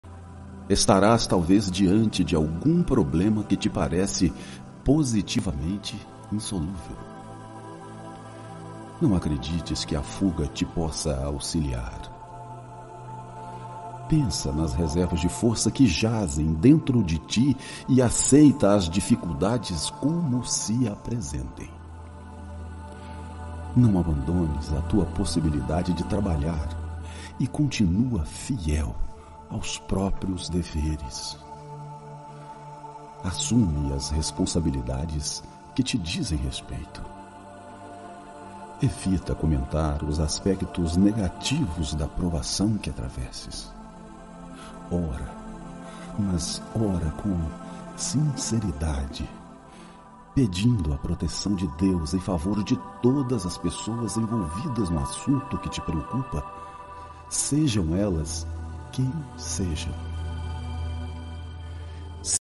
ensinamentos de Chico Xavier para refletir
Francisco Cândido Xavier, mais conhecido como Chico Xavier, foi um médium, filantropo e um dos mais importantes expoentes do espiritismo. Chico Xavier escreveu mais de 450 livros, que até o ano de 2010 já haviam vendido mais de 50 milhões de exemplares.